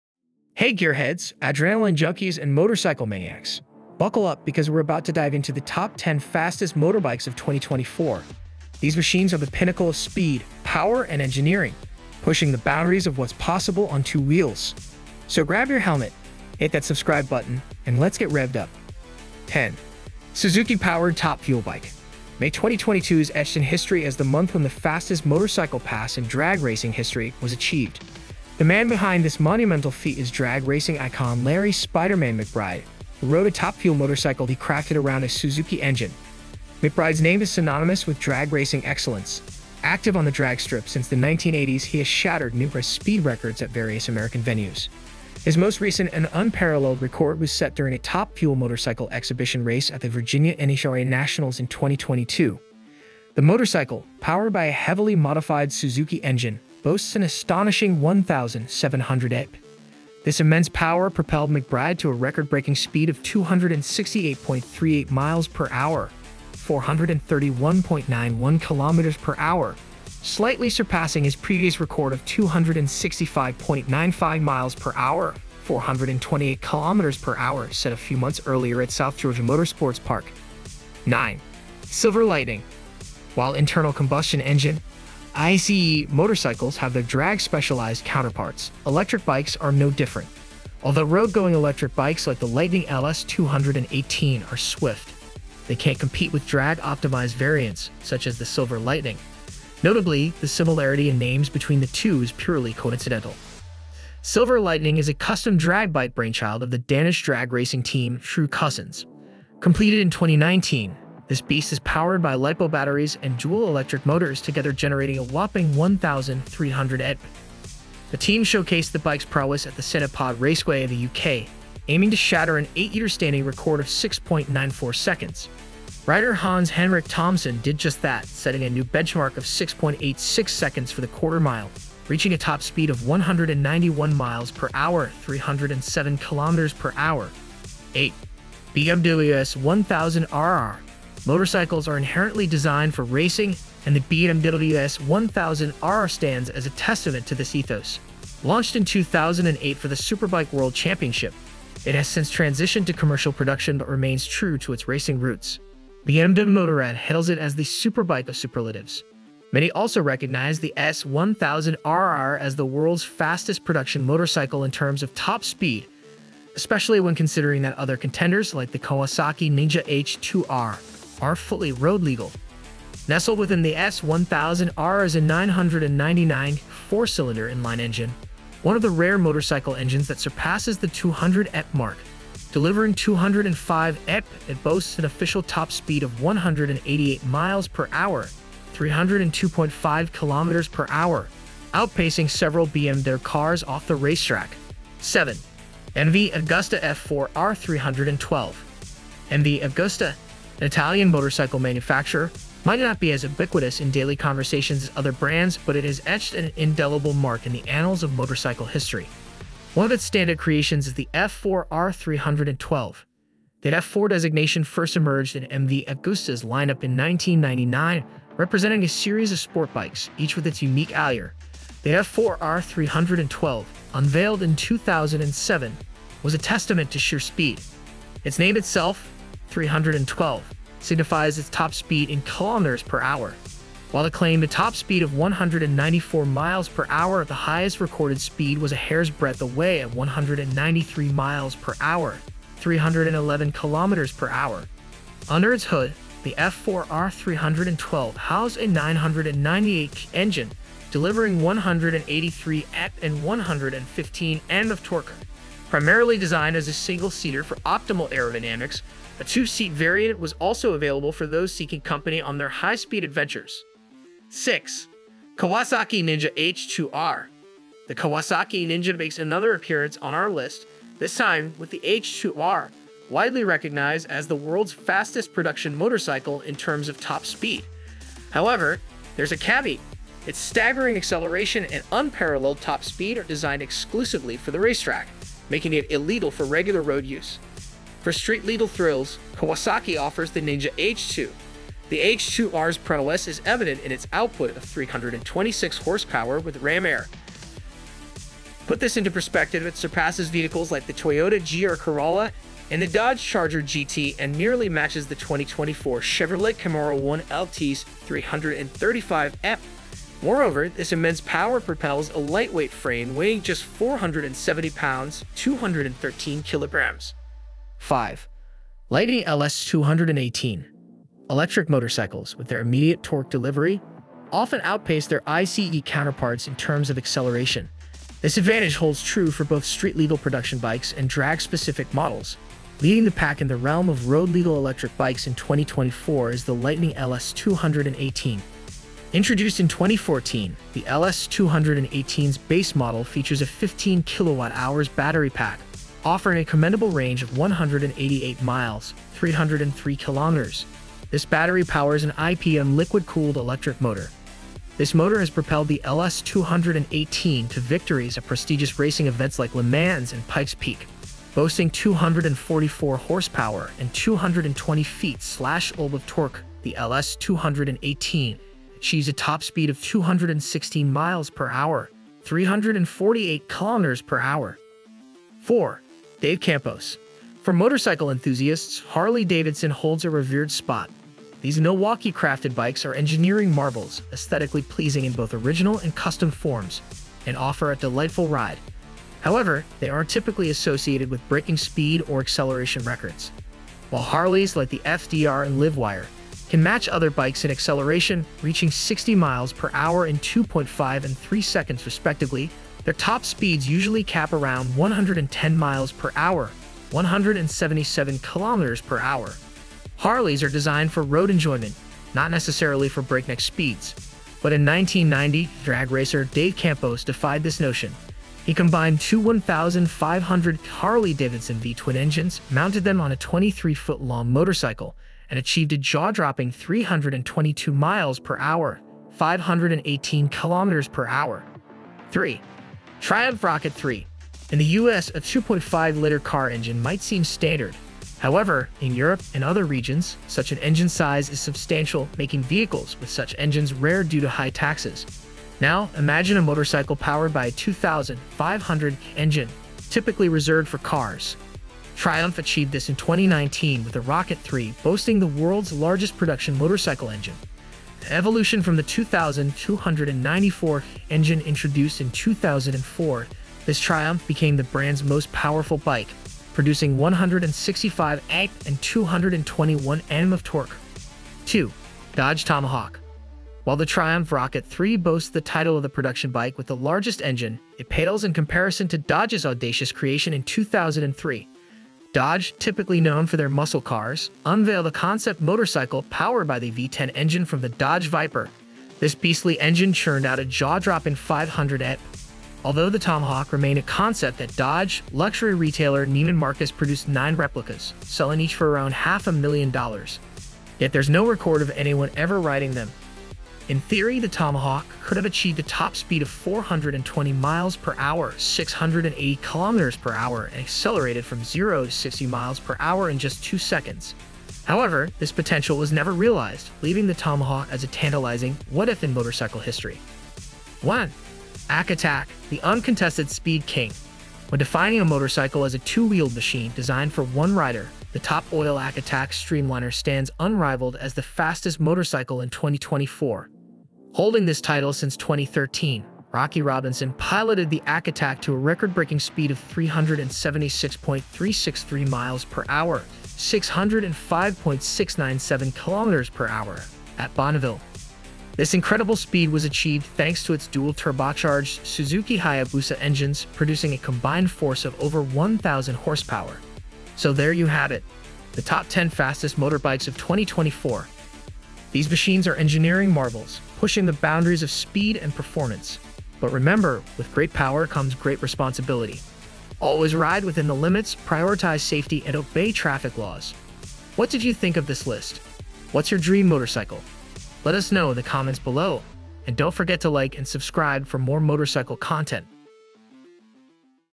TOP 10 Fastest Bikes in the World 2024.
the top 10 fastest motorbikes of 2024! These machines are engineering marvels, pushing the boundaries of speed and performance. But remember, with great power comes great responsibility. Always ride within the limits, prioritize safety, and obey traffic laws.
What did you think of this list? What's your dream motorcycle?

Disclouser👇
Top Valid Tech is a participant in the Amazon Services LLC Associates Program. I earn through amazon associates from qualifying purchases.